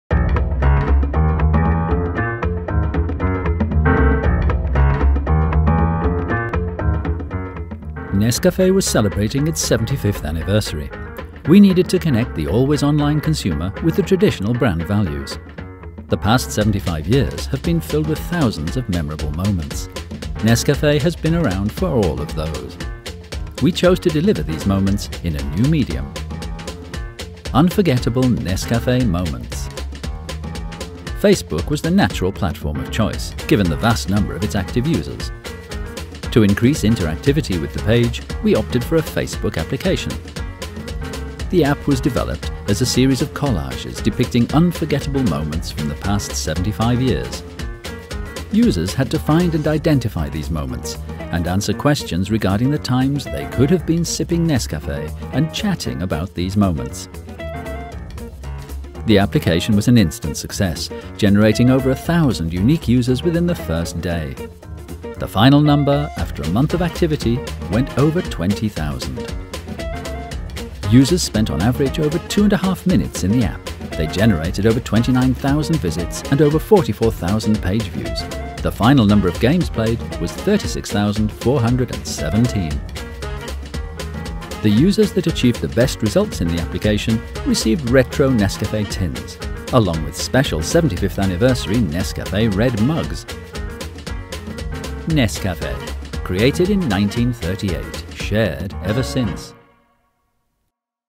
Nezaboravni NESCAFÉ trenuci - Case Study by Drap
A Case Study video for NESCAFÉ's Facebook celebration of its' 75th anniversary.